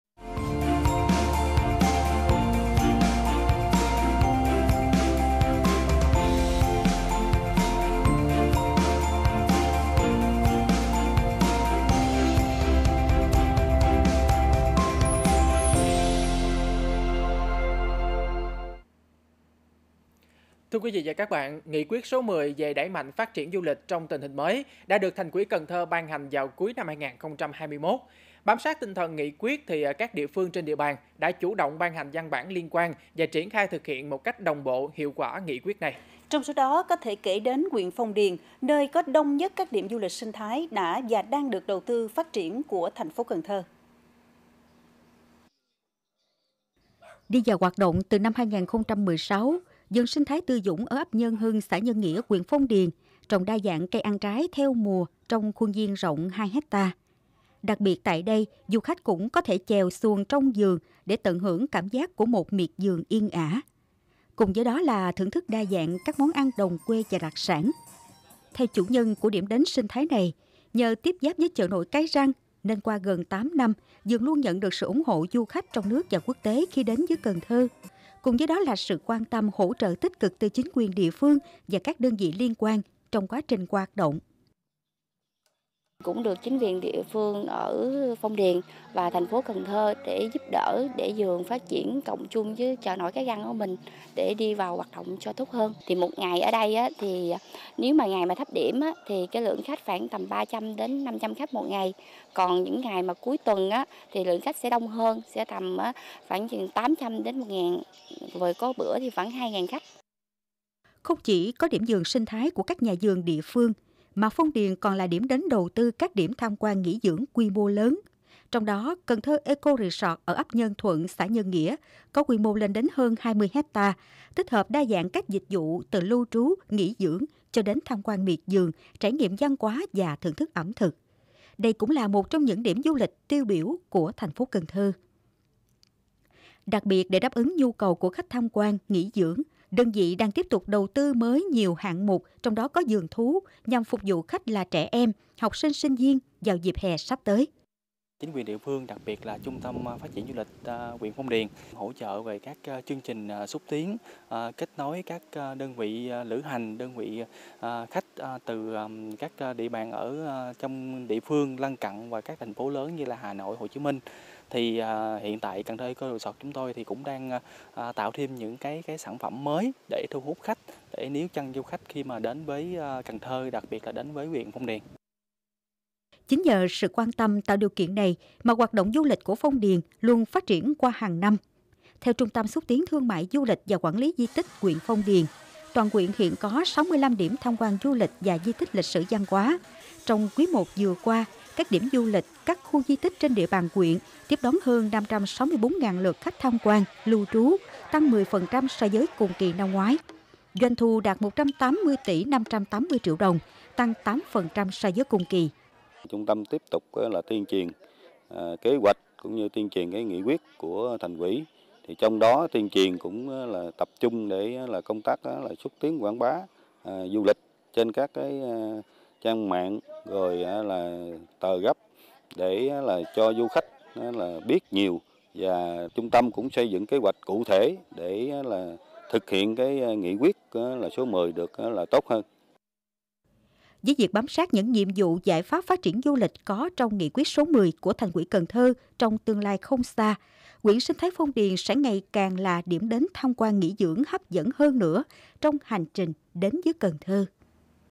Phong Điền phát triển du lịch trong tình hình mới | Cần Thơ TV
Nghị quyết số 10 về đẩy mạnh phát triển du lịch trong tình hình mới đã được Thành uỷ Cần Thơ ban hành vào cuối năm 2021. Bám sát tinh thần Nghị quyết, các địa phương trên địa bàn đã chủ động ban hành văn bản liên quan và triển khai thực hiện một cách đồng bộ, hiệu quả Nghị quyết này. Trong số đó, có thể kể đến huyện Phong Điền – nơi có đông nhất các điểm du lịch sinh thái đã và đang được đầu tư, phát triển của TP. Cần Thơ.